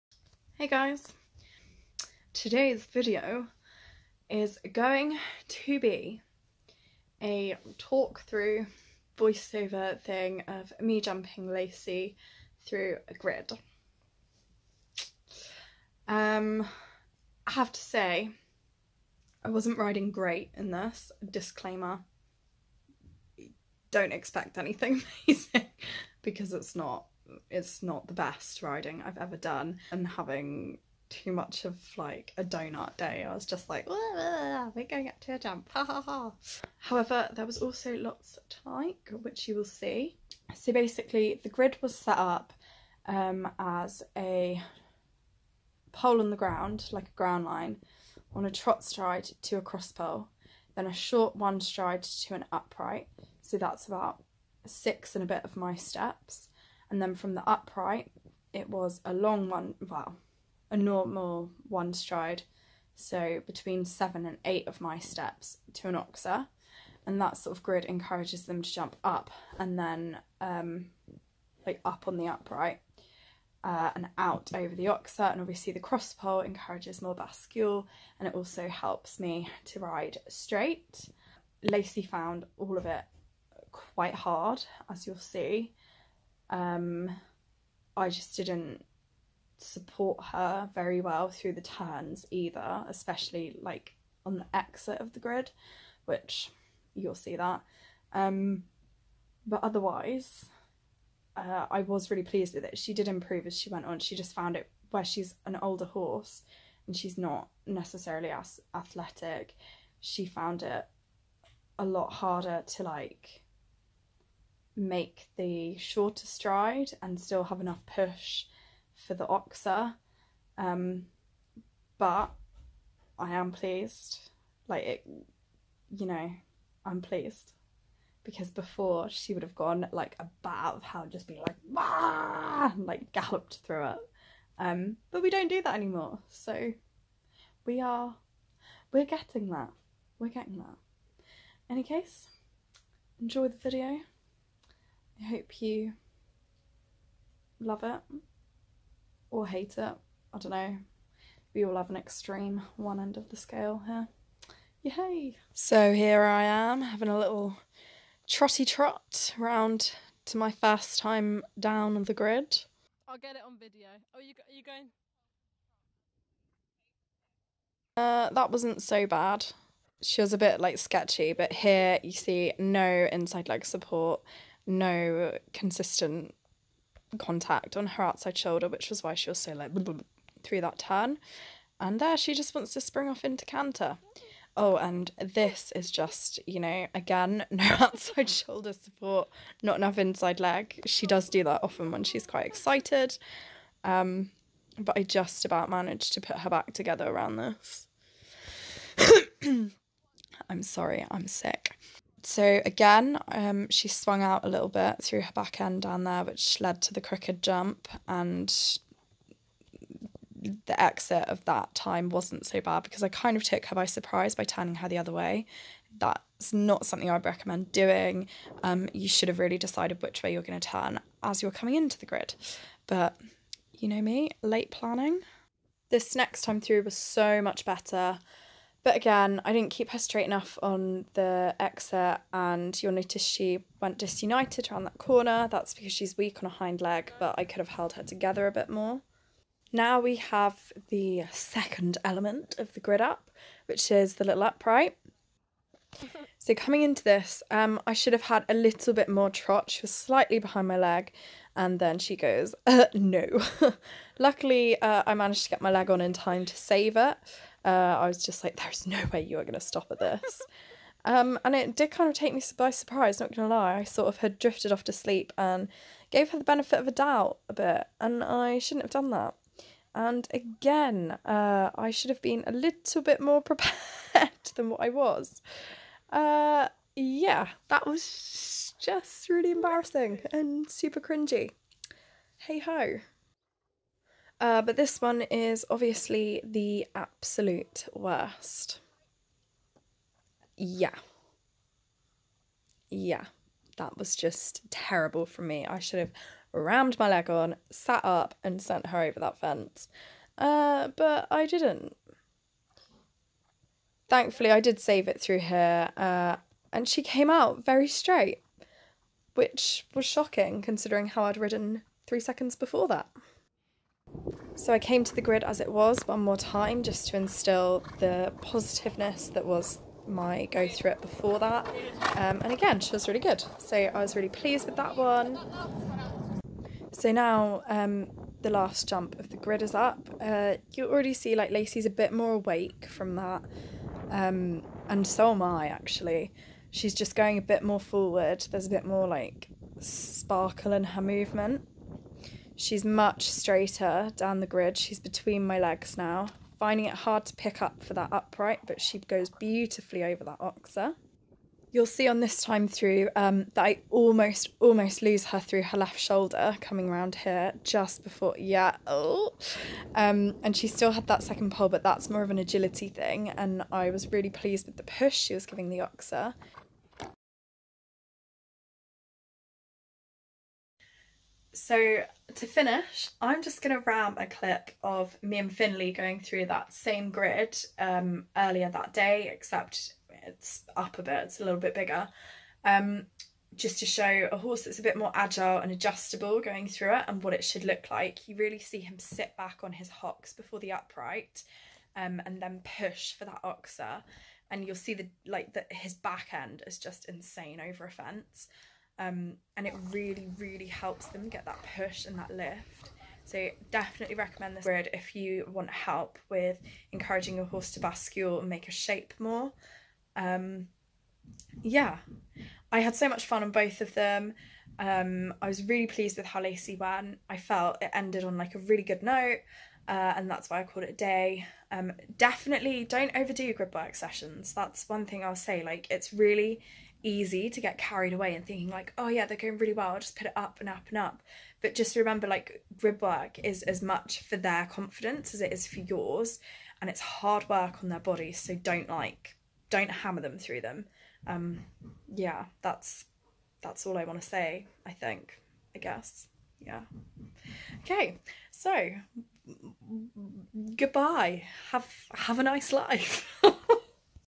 Gridwork // Improving your horses jump
The mistakes were plentiful, the bad riding is ceaseless but Lacey has a heart of gold 🧡🧡

HORSES:
Lacey - 16hh, ISH piebald mare, 19yo.
Finley - 16.2hh warmblood gelding, 10yo.